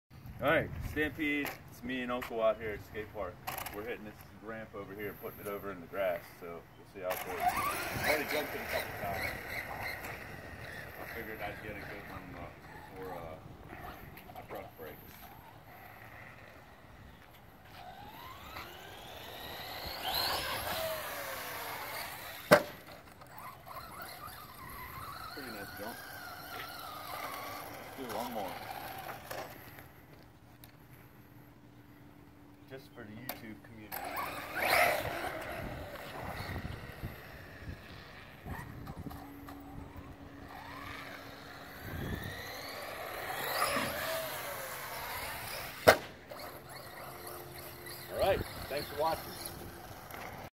Stampede skatepark jump
Just a short clip of the stampede jumping the ramp at the park.